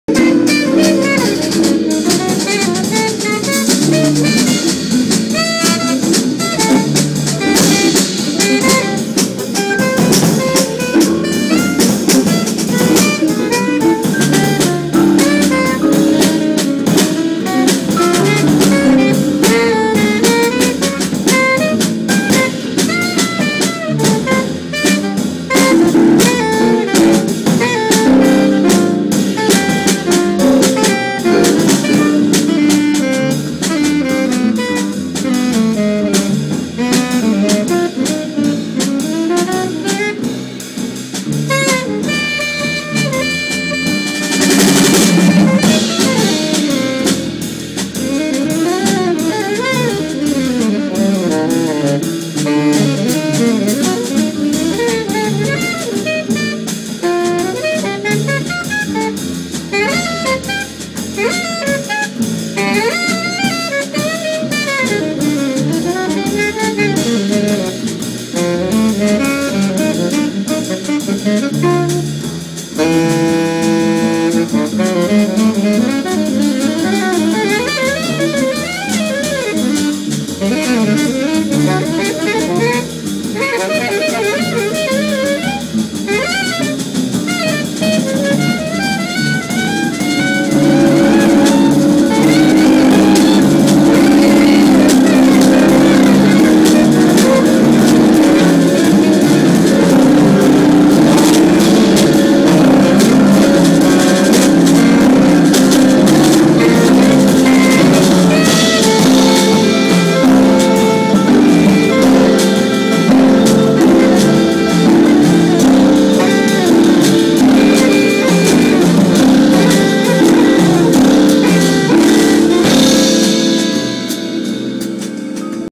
Matt Blostein @ North Sea Jazz Festival
Matt Blostein with Liam Sillery Quintet @ North Sea Jazz Festival